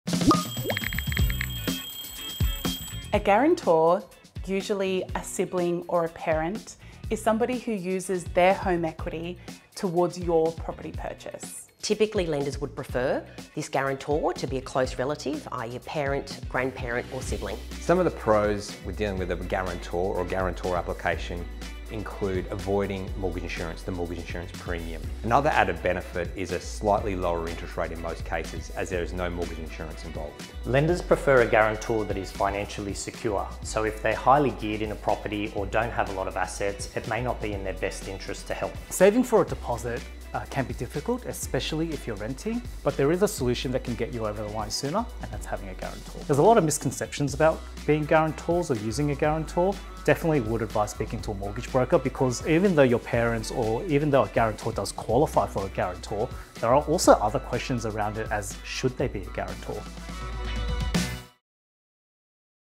What is a guarantor?
Saving for a deposit can be difficult but there is a solution that could get you over the line sooner - that's having a ‘guarantor’. Our brokers explain how a guarantor can help you purchase your first home.